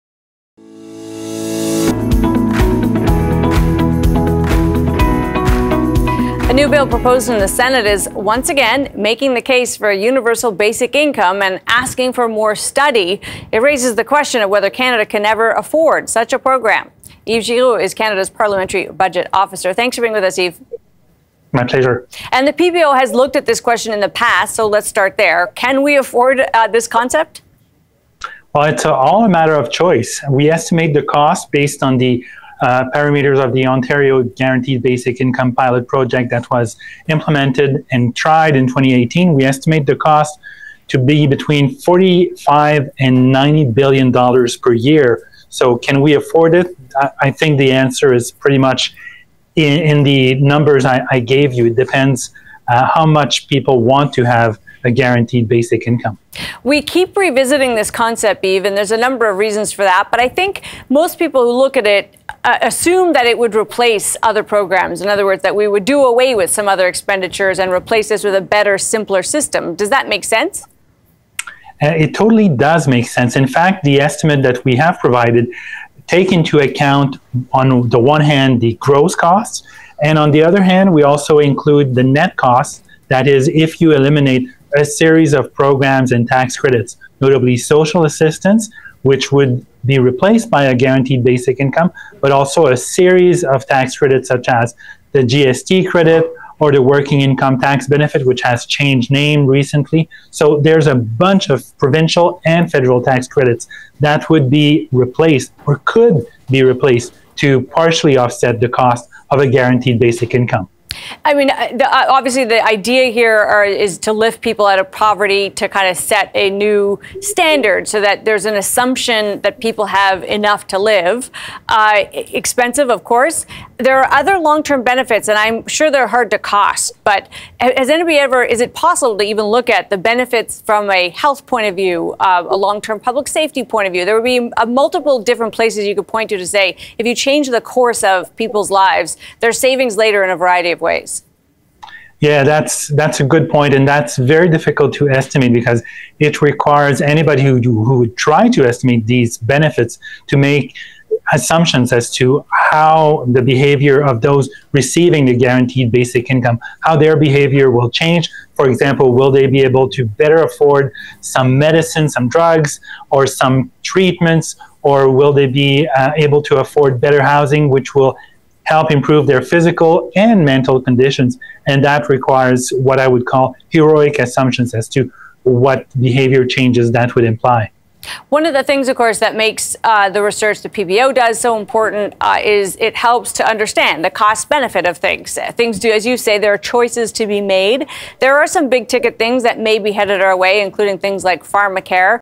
Taking Stock - Another kick at the guaranteed income can in Canada
Takeaway - The surge in international students isn't just about housing, it highlights the degree to which our universities and colleges are shifting from being universal, tax-payer funded places accessible to all Canadians. Maybe that's fine, but shouldn't we talk about it?

Guest: Yves Giroux, Parliamentary Budget Officer